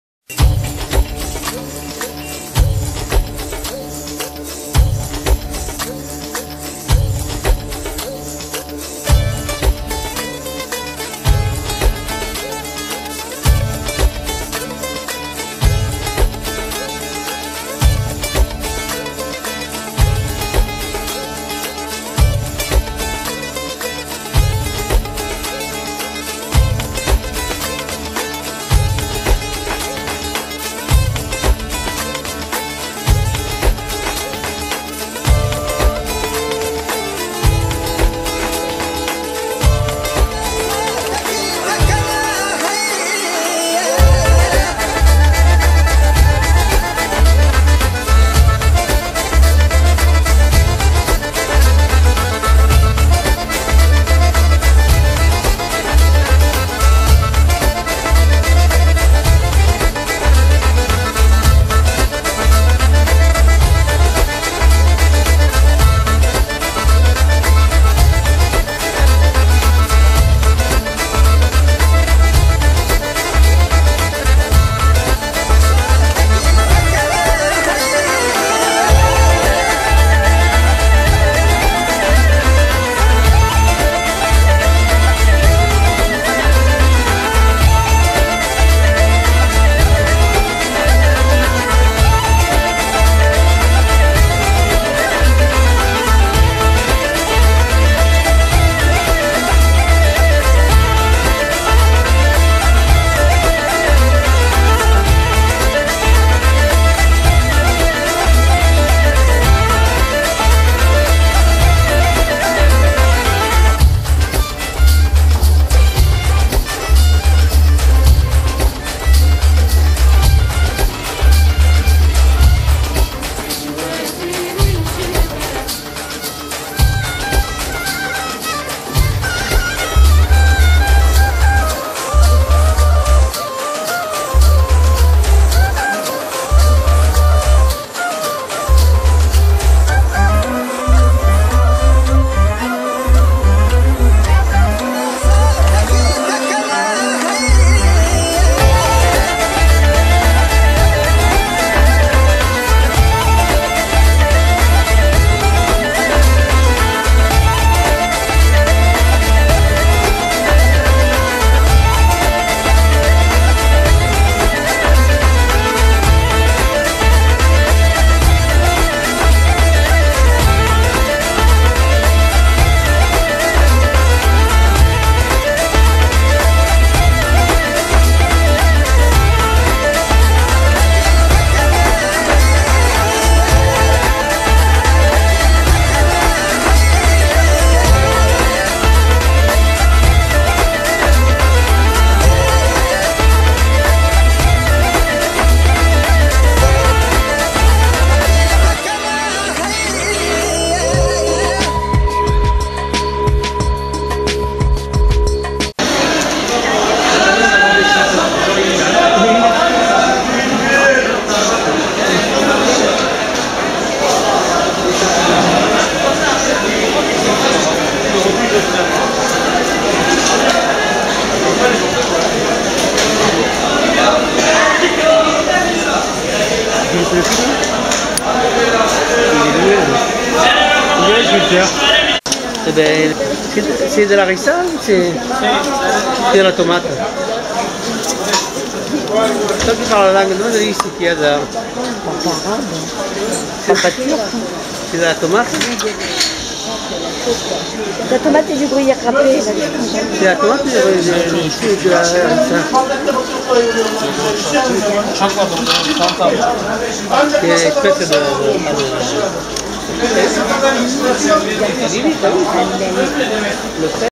Le marché couvert d'Antalya
En fin de vidéo, vous verrez avec quelle rapidité le Turc épluche un artichaut pour n'en garder que le coeur...!